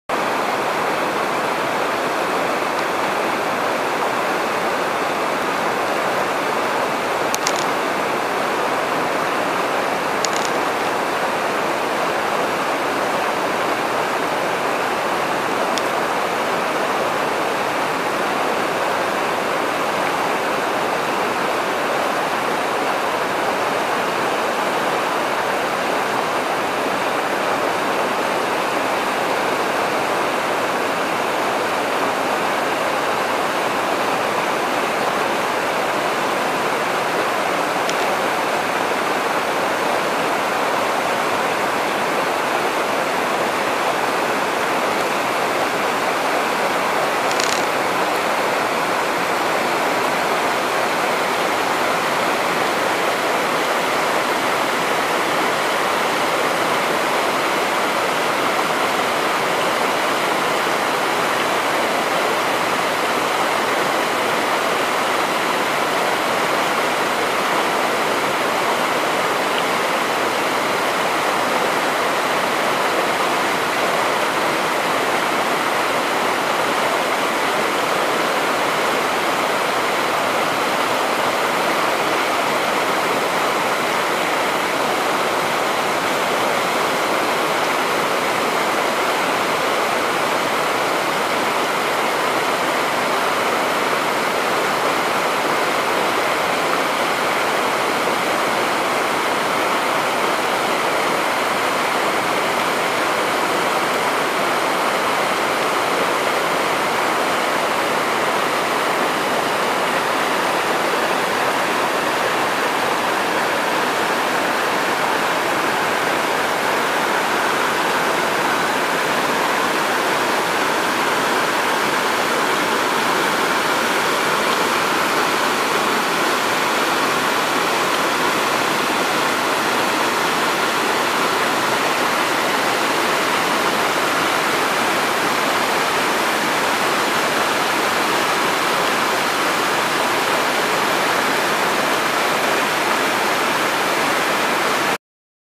Mannapaapu mane 2
Identify the Frogs - workshop conducted by Dr. KV Gururaja @ Mannapaapu mane, Maala, Udupi dt. 23 & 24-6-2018